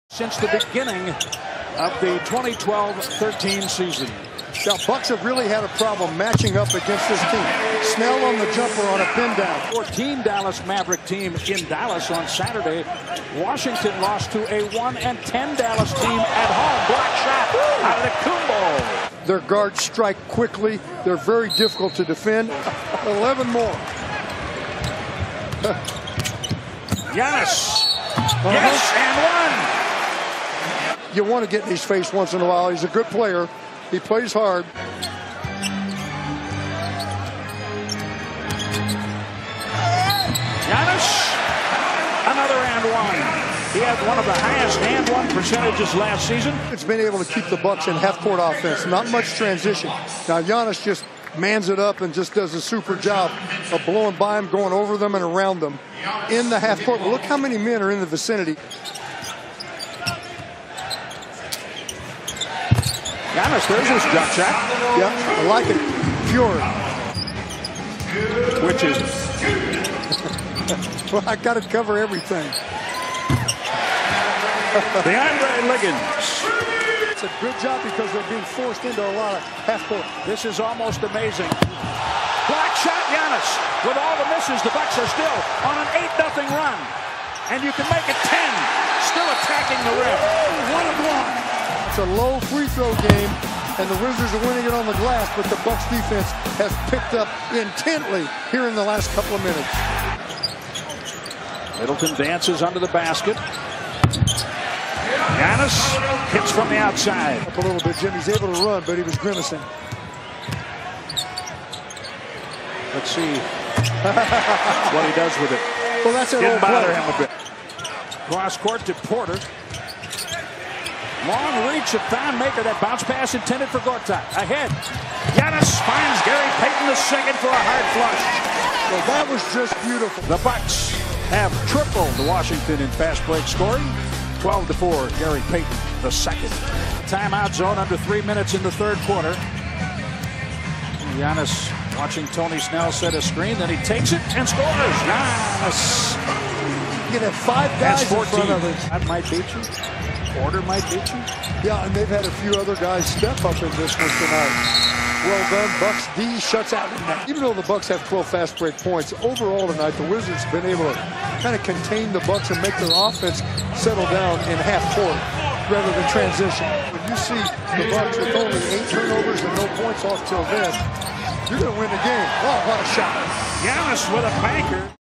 Giannis Antetokounmpo 23 points vs. Wizards [Full Highlights] | 11.20.17
The best from Giannis Antetokounmpo tonight against the Wizards:
23 pts | 8 reb | 4 ast | 3 bks